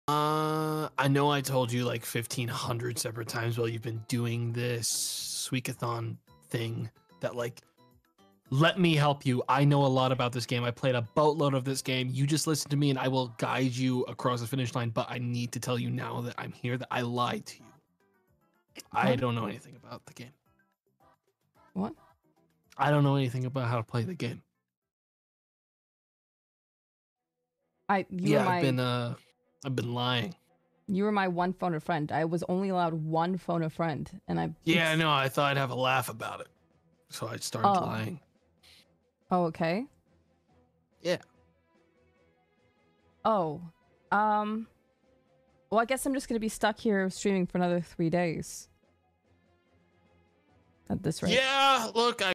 LaynaLazar: Tomato is No Help
Nothing like finding out the friend you were counting on to coach you was BS'ing you the whole time. But with Tomato, not particularly surprising.